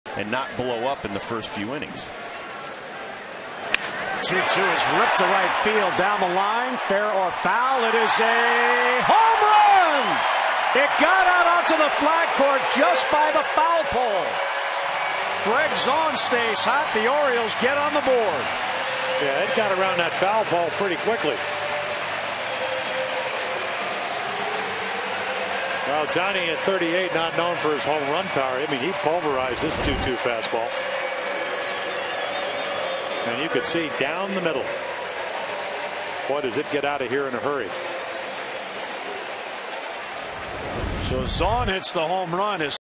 2009/06/27 Zaun zings one out
WSH@BAL: Zaun jacks a solo homer to right field